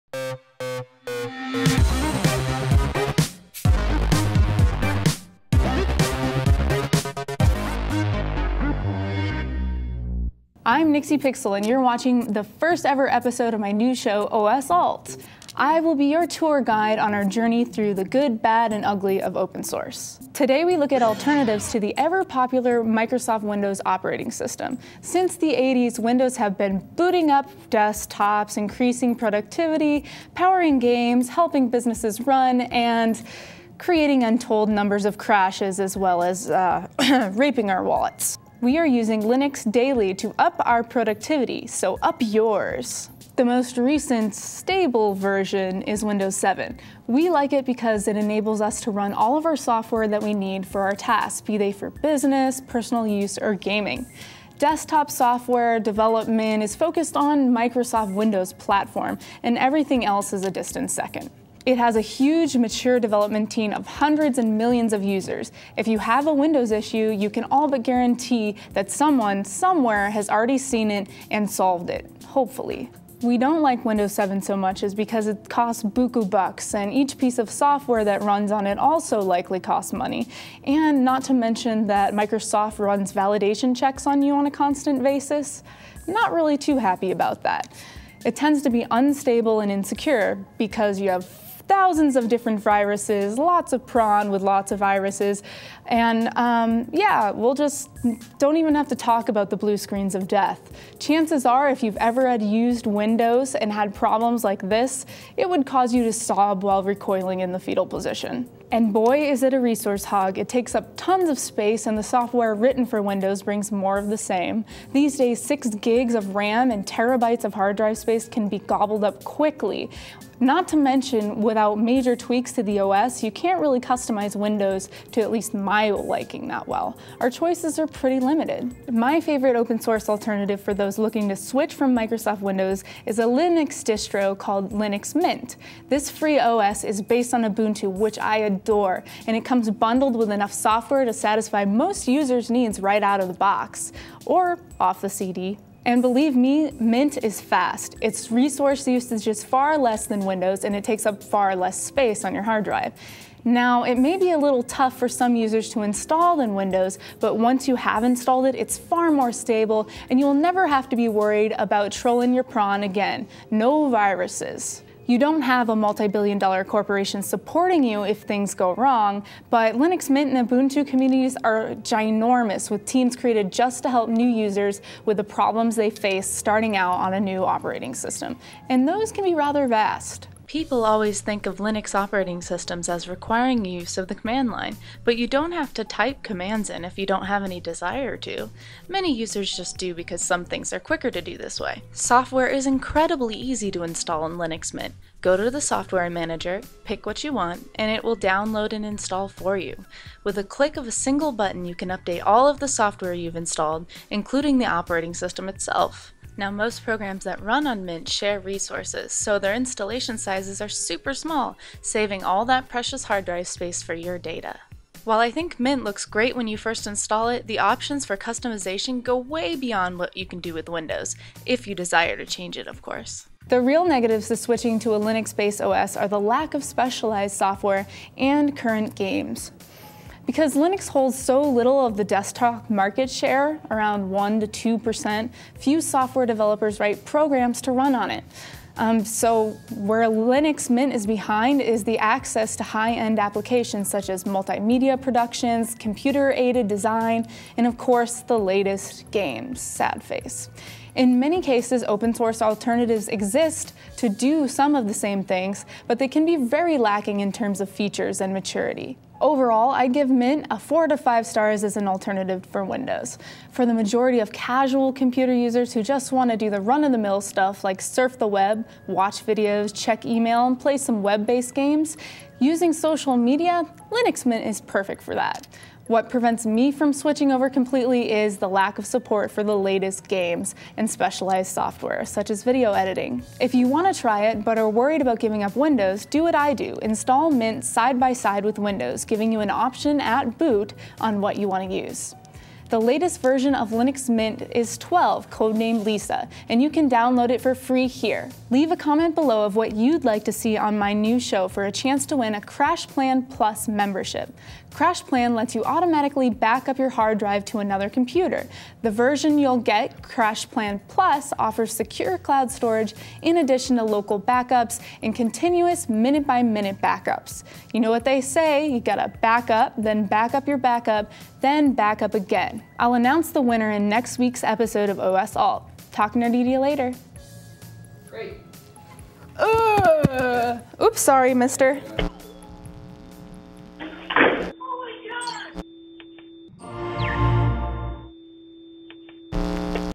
Linux VS Windows
For my first episode I compare Linux vs Windows! Watch me tackle my favorite alternative for those using Microsoft Windows: Linux Mint. For the vast majority of desktop & laptop computer users, Linux Mint is a great switch from the proprietary (and expensive!) Windows... but not always.